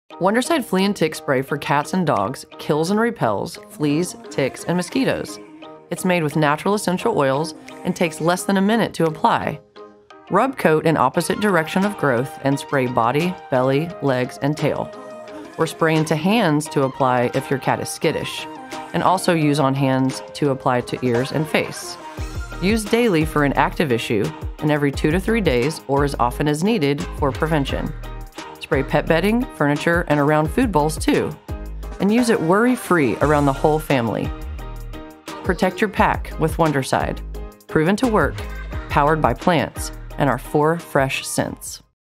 Wondercide Flea & Tick Spray for Dogs & Cats – with Natural Essential Oils
Flea & Tick Spray for Pets + Home with Natural Essential Oils

Proven to work and easy to use. This plant-based pest protection kills, repels, and prevents fleas, ticks, mosquitoes, flies, and hundreds more pesky bugs. Safe for dogs and cats of all ages when used as directed.

Kills, repels & prevents
fleas, ticks, and mosquitoes.

Safe & effective when used as directed
A plant-based alternative to conventional drops, pills, and collars. Eliminates pests and provides lasting repellency.

Treat Nature with Nature™
Made with steam-distilled essential oils and other good things from Mother Nature. No artificial colors or fragrances. Clove-free.

Mom made. Mom approved.
Handcrafted from plant-based, sustainable ingredients.